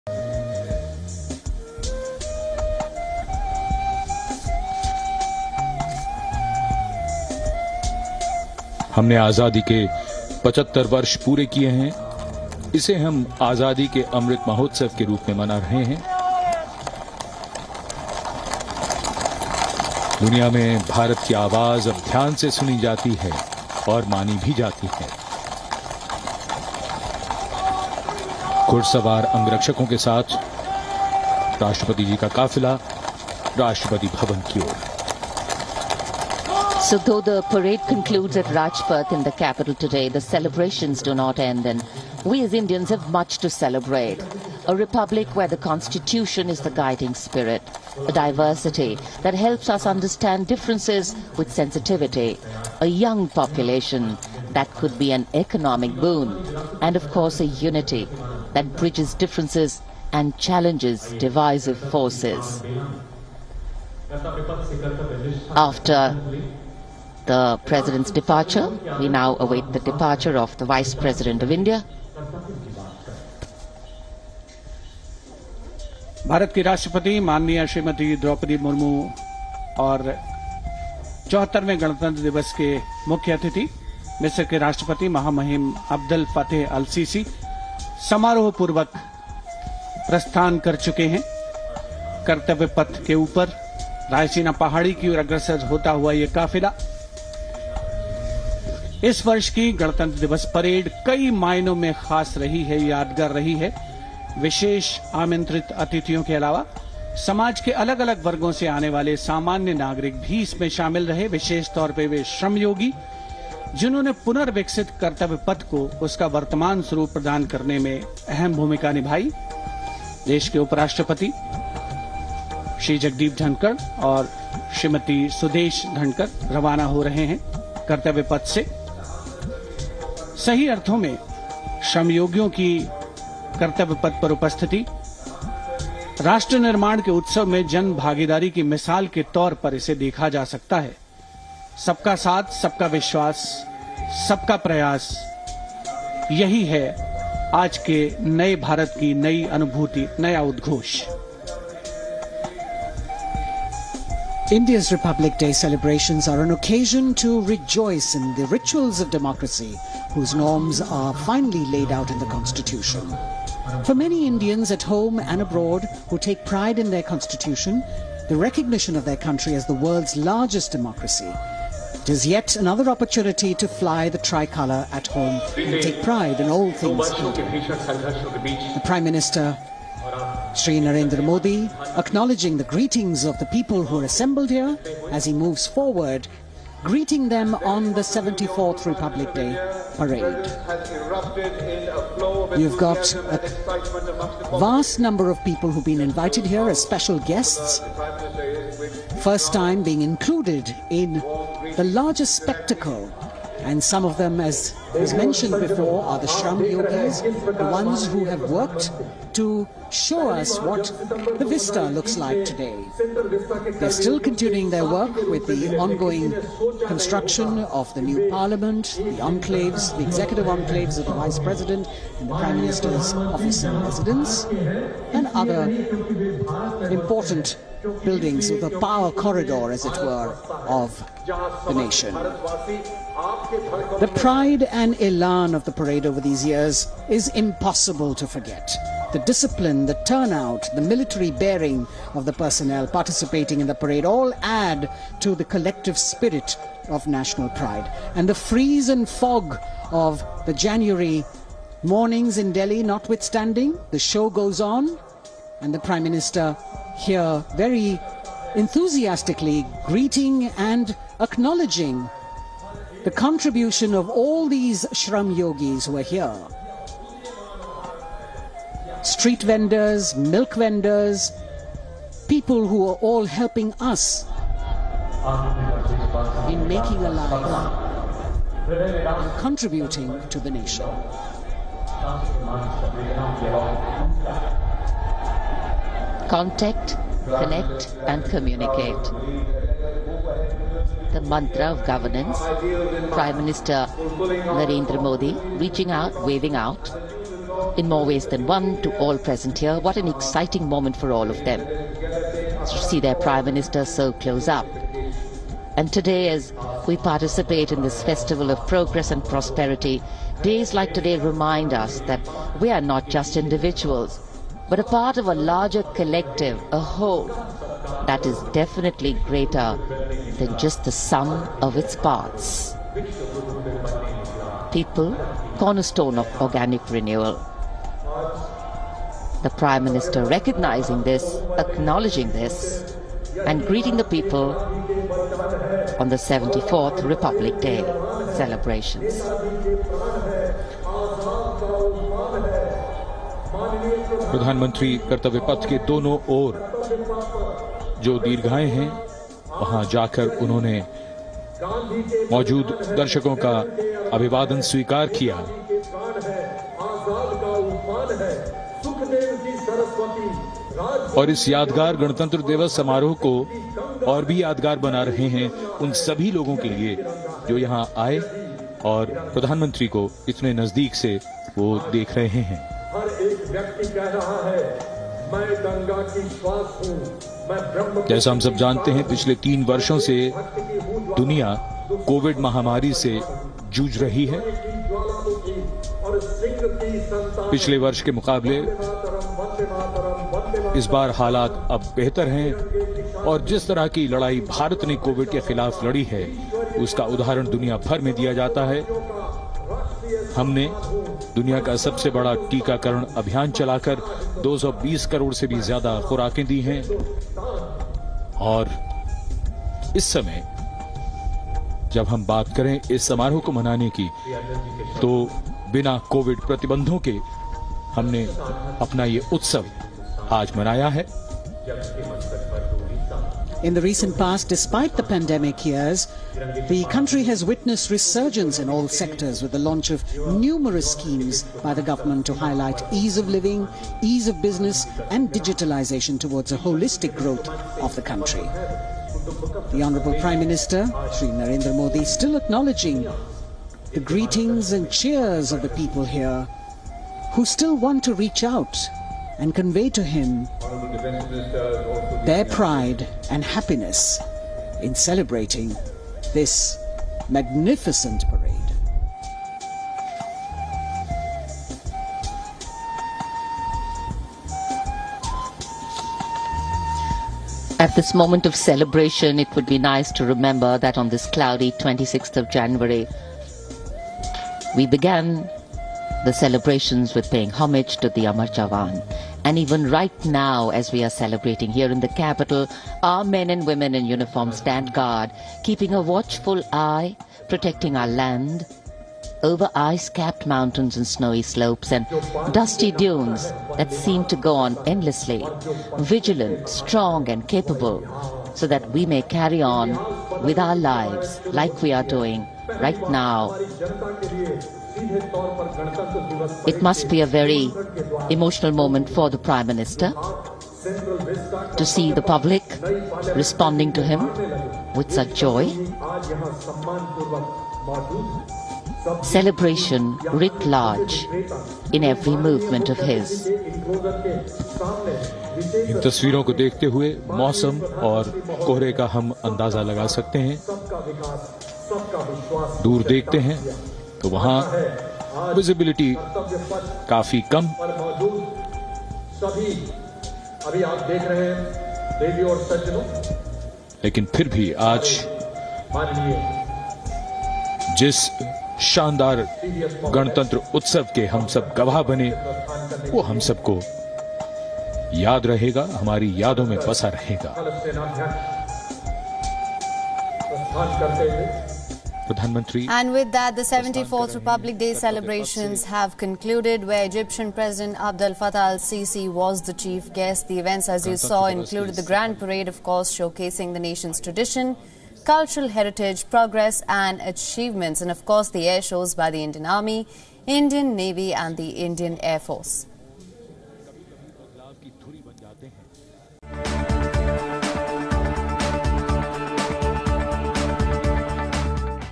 India Republic Day 2023: NCC, and NSS march at the 74th republic day parade
India is celebrating its 74th Republic Day today. The main function was held at Kartavya Path in New Delhi, where President Droupadi Murmu led the nation in celebrating Republic Day. Egyptian President Abdel Fattah El-Sisi was the Chief Guest at the parade. During the Republic Day parade, the country’s military prowess, cultural diversity, self-reliance initiatives, women empowerment, and emergence of a New India were showcased.